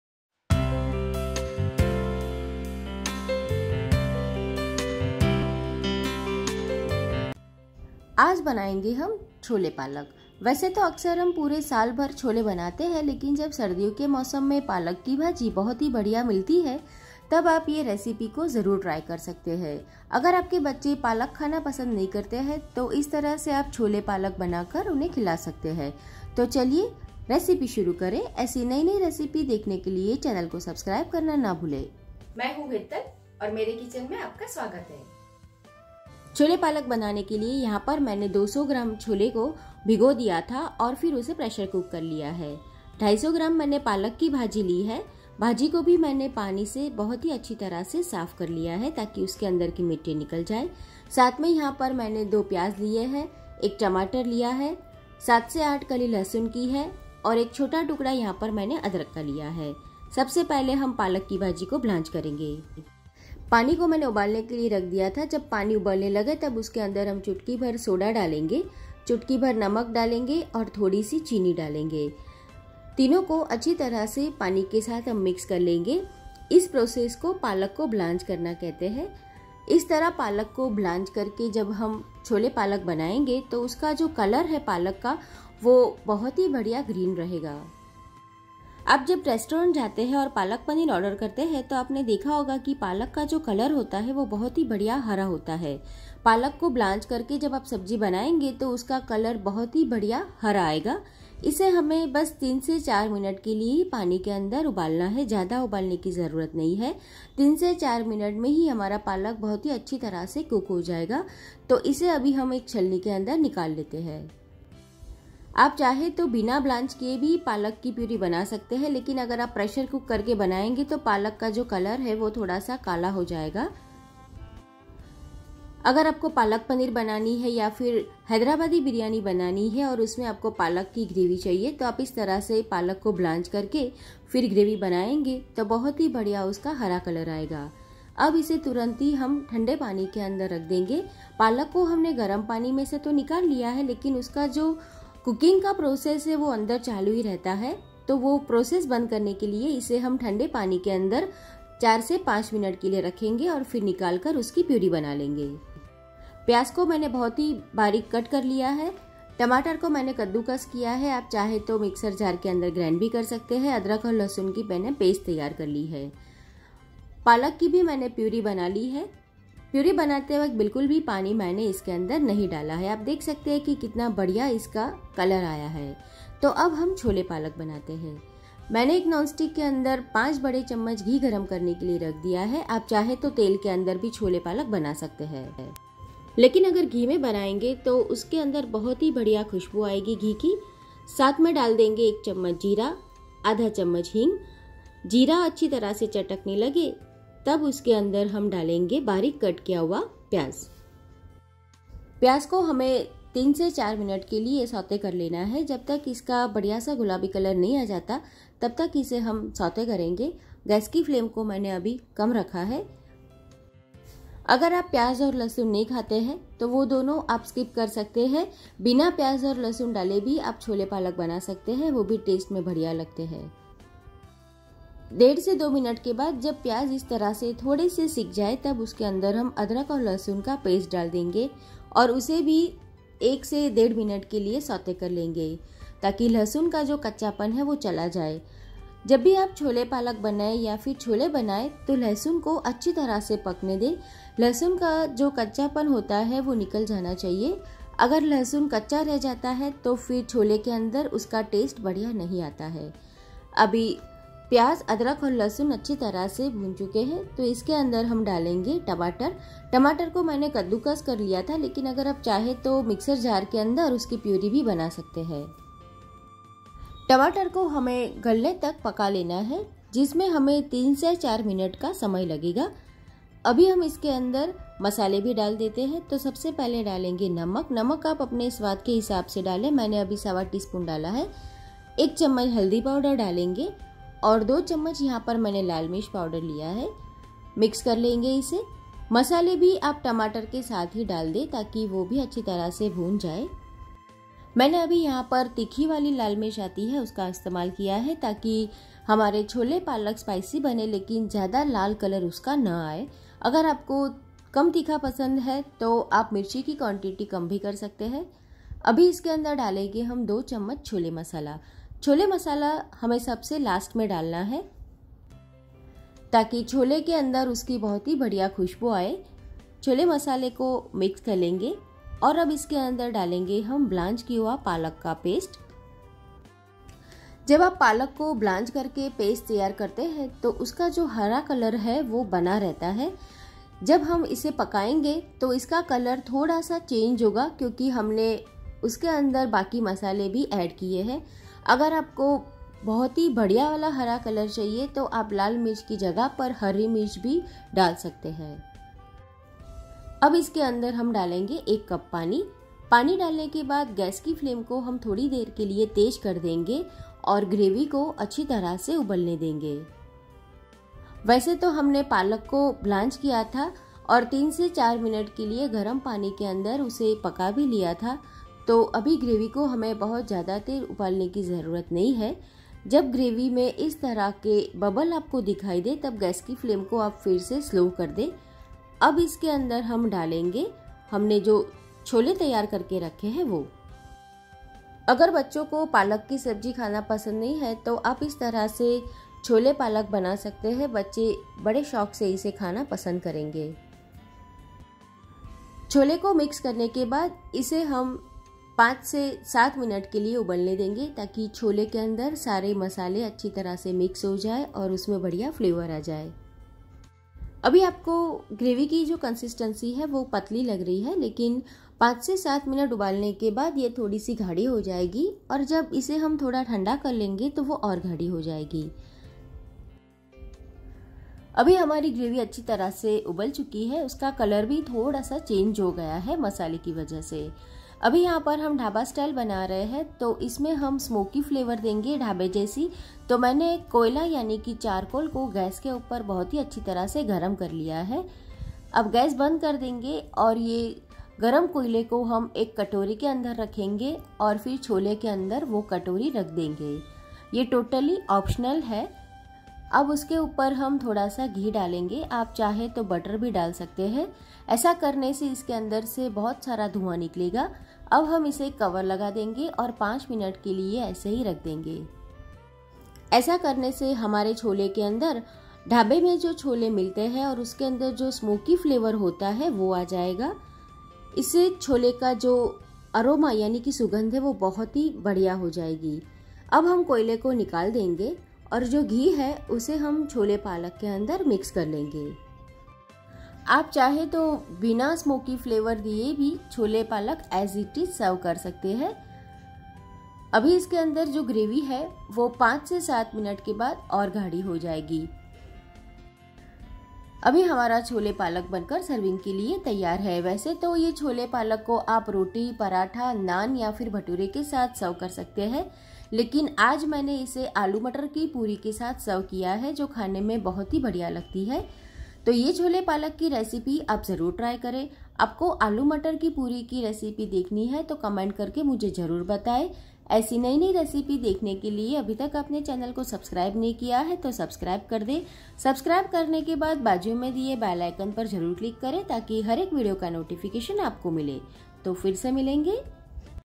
chole recipe | हलवाई स्टाइल छोले बनाने के सारे राज़ जानिए आज | chole palak recipe| Dhaba style chole
INGREDIENTS
250 grams spinach [ palak ]
200 grams boil chole
2 to 3 medium size onion
2 tomato
7 to 8 garlic clove
1 small piece of ginger
4 tbs ghee
1 ts cumin seeds
1/2 ts hing
1 ts turmeric powder
2 tbs red chilly powder
2 ts chole masala